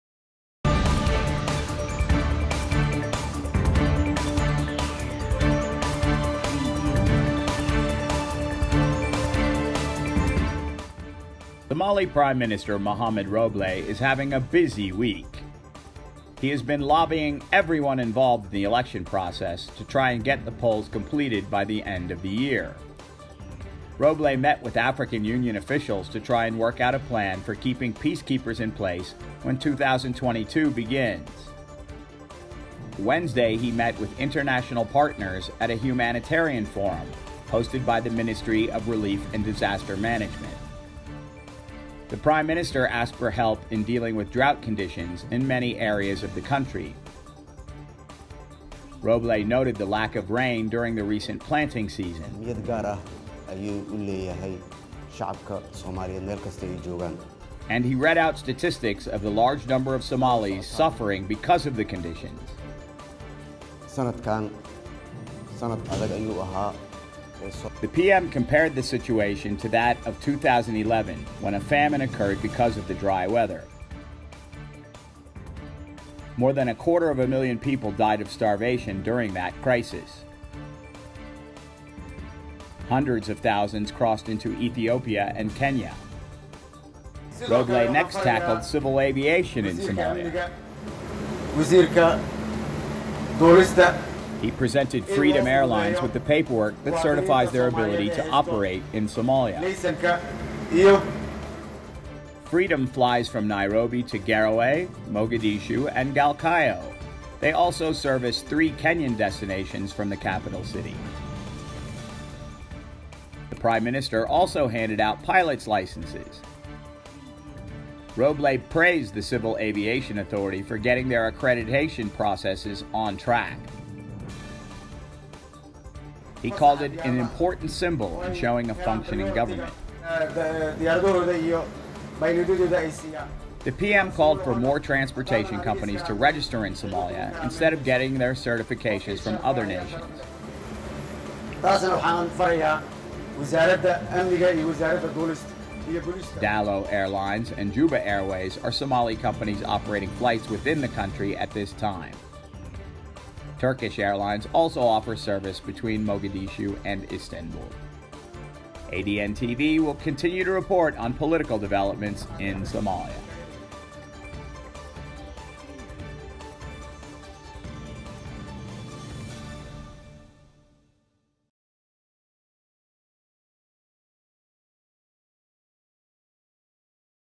ROBLE BUSY WITH DROUGHT AND CIVIL AVIATION AS WELL AS ELECTIONS.
Somali Prime Minister Mohamed Roble is having a busy week. He has been lobbying everyone involved in the election process to try and get the polls
completed by the end of the year. Roble met with African Union officials to try and work out a plan for keeping peacekeepers in place when 2022 begins. Wednesday he met with international partners at a Humanitarian Forum, hosted by the Ministry of Relief and Disaster Management.